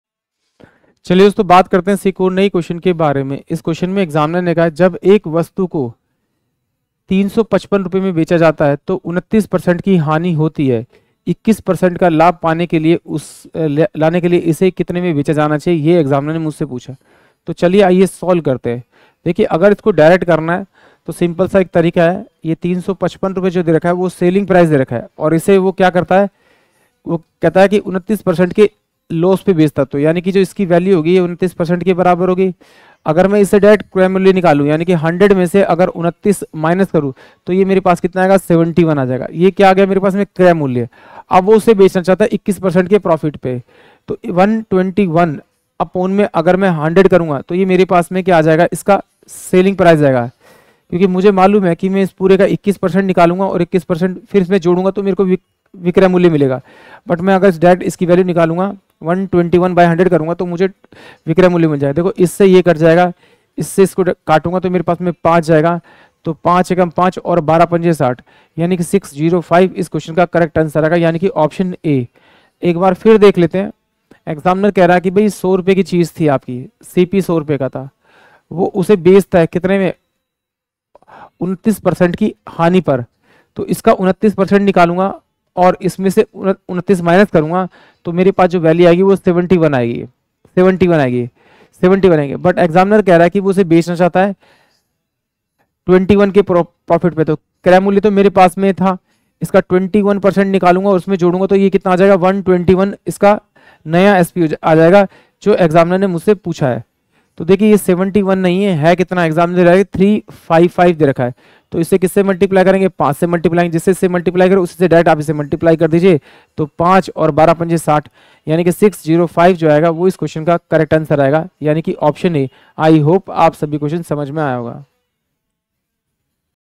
When an article is sold for 355, there is a loss of 29%. To gain 21%, it should be sold for
जब एक वस्तु को 355 में बेचा जाता है, तो 29% की हानि होती है। 21% लाभ पाने के लिए, इसे : में बेचा जाना चाहिए:
605
629.20
635
580.80